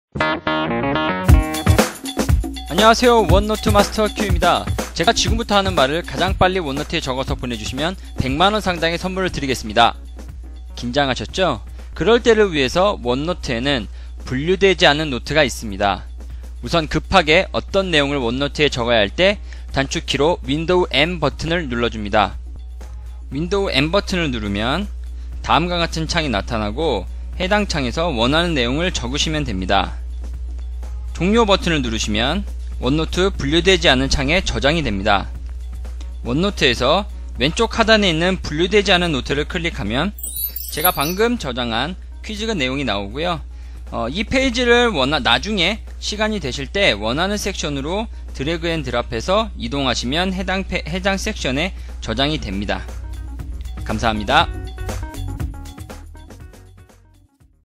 [OneNote2010] 원노트 30일 작전 D+16 분류되지않은노트
간단한 필기를 위해 분류되지 않은 노트를 활용법을 알아봅니다.